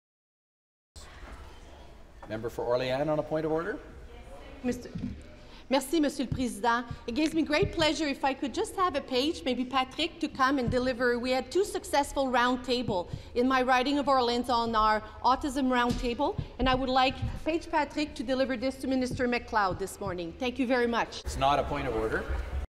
Presenting findings from my autism round tables to the Minister
Marie-France Lalonde Orléans